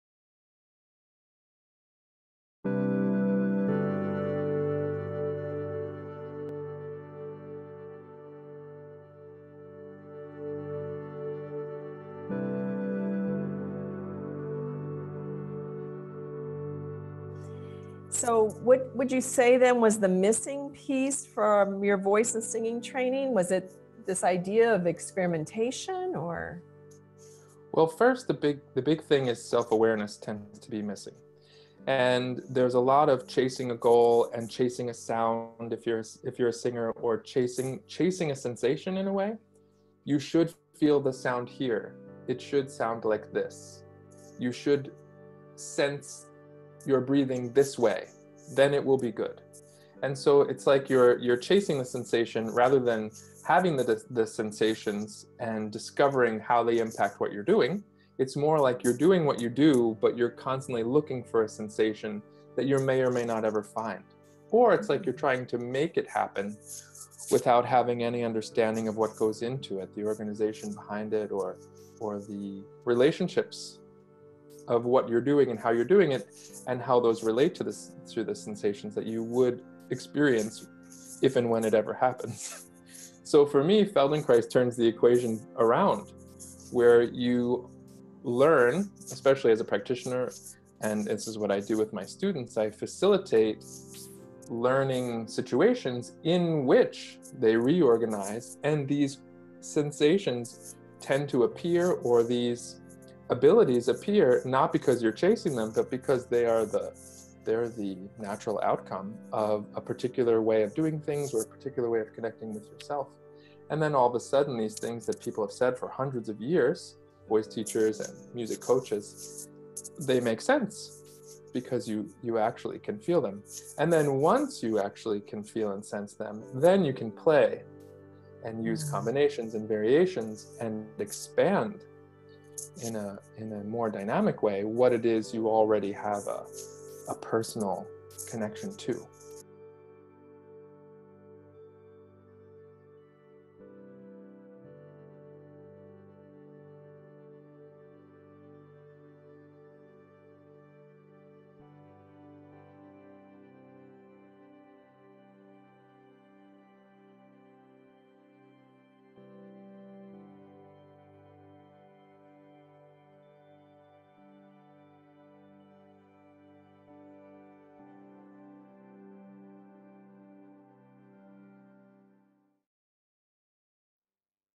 Voice, Singing and Feldenkrais
Robert Sussuma, voice and Feldenkrais teacher shares how striving to hit that perfect note can be left behind with a somatic approach to learning and singing. Get this full interview that includes a rich somatic exploration along with 24 other interview from the Feldenkrais Awareness Summit at futurelifenow-online.comf